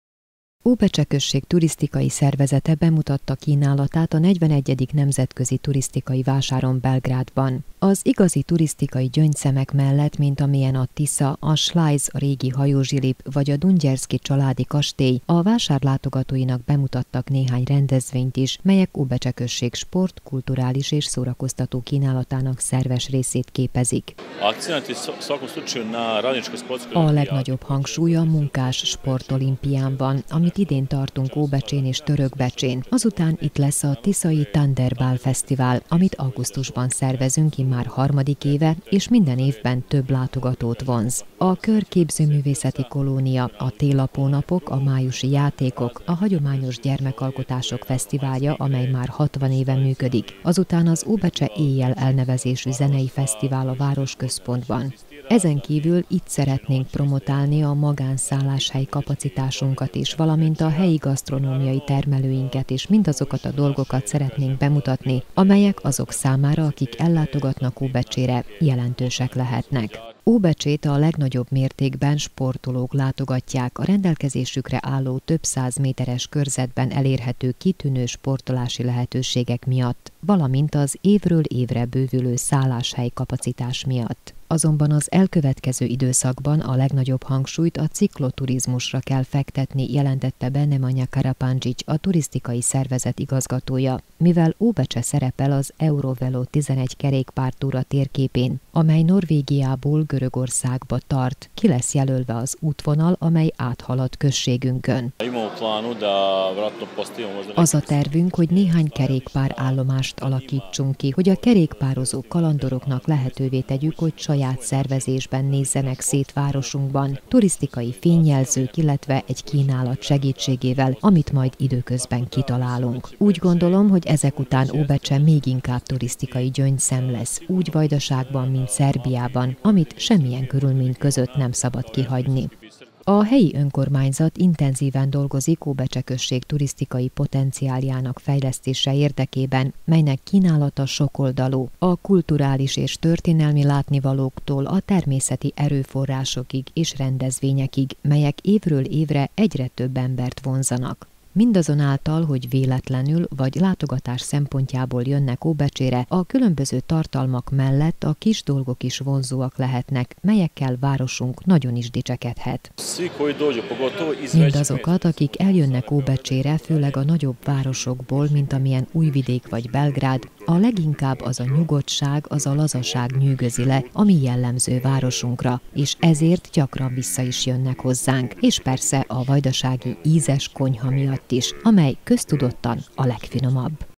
TV BEČEJ: Az óbecsei rendezvények kitűnő „csalik” a turisták számára
TV BEČEJ: Óbecse község Turisztikai Szervezete bemutatta kínálatát a 41. Nemzetközi Turisztikai Vásáron Belgrádban.